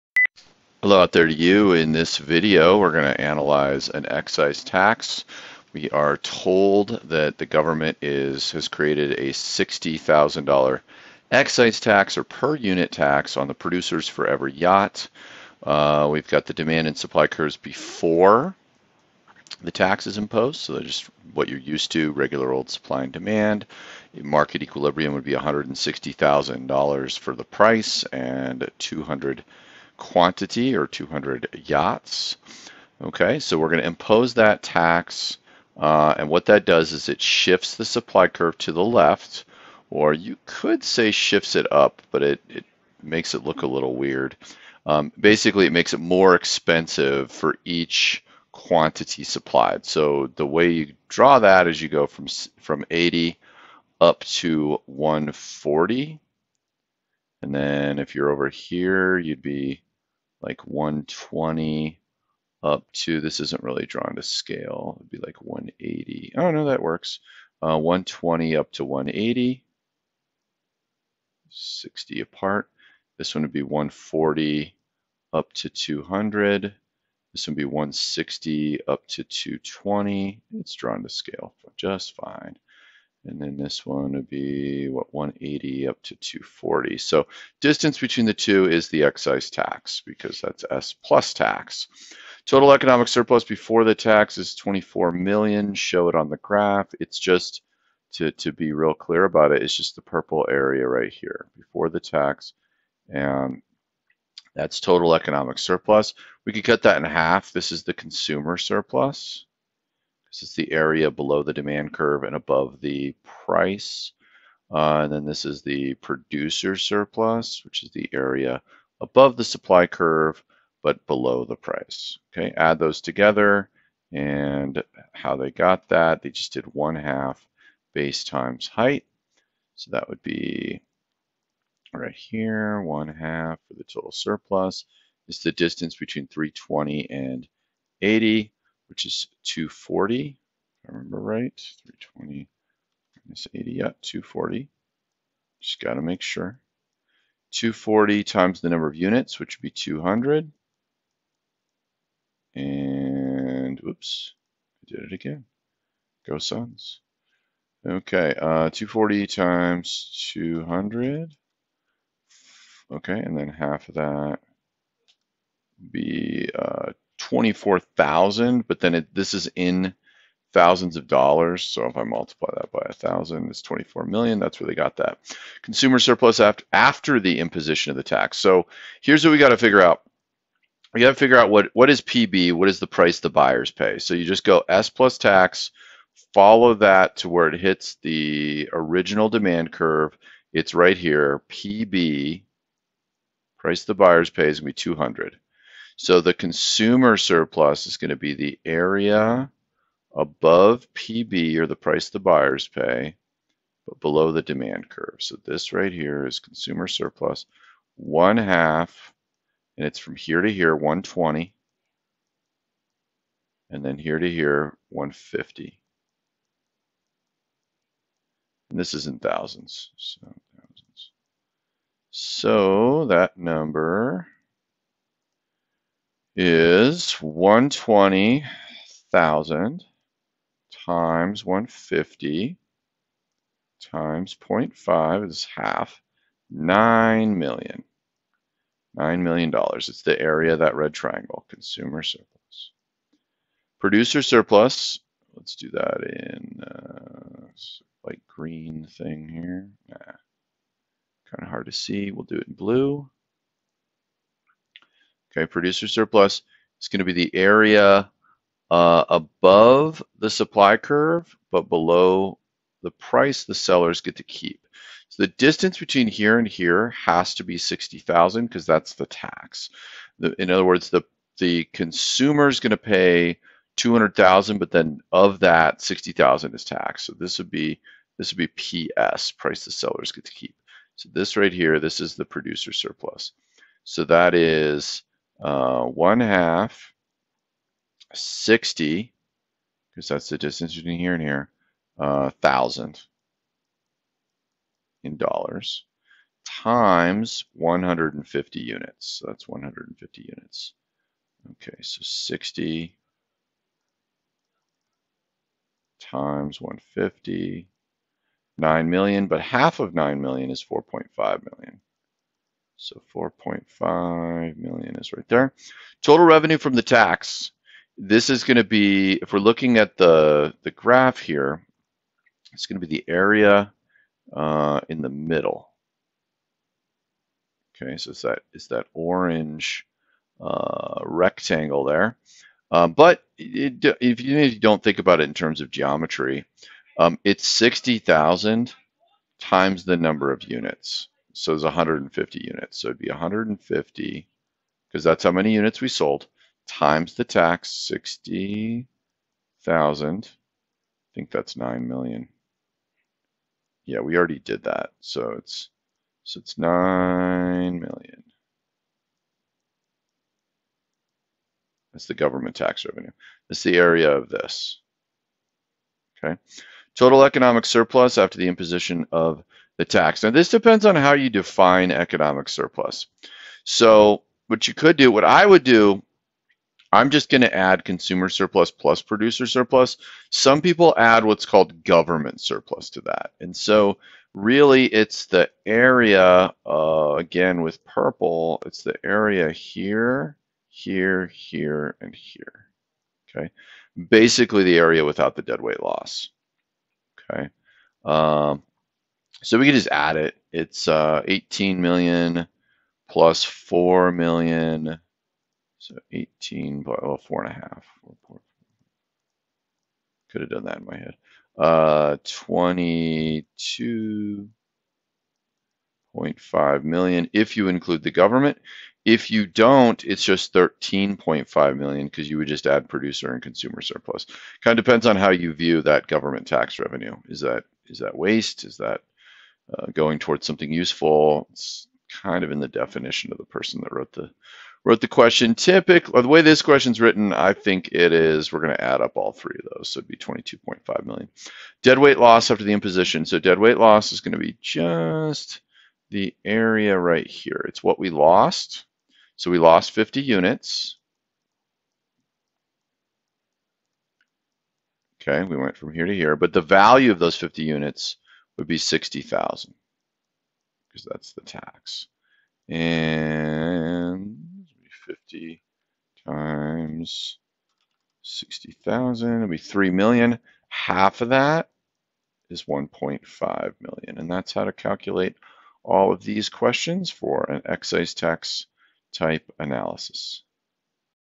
How to calculate tax revenue and dead weight loss after an excise tax on yachts
A tax on yachts would normally be considered a tax on wealthy consumers. Consider a $60,000 excise tax on producers for each yacht sold. The graph illustrates the demand and supply curves for yachts both before and after the tax is imposed.

a. Total economic surplus before the tax is $24 million.
Show this area on the graph.
b. What is the consumer surplus after the imposition of the tax?
c. What is the producer surplus after the imposition of the tax?
d. What is the total revenue generated from the tax?
e. What is the total economic surplus after the imposition of the tax?
f. What is the deadweight loss after the imposition of the tax?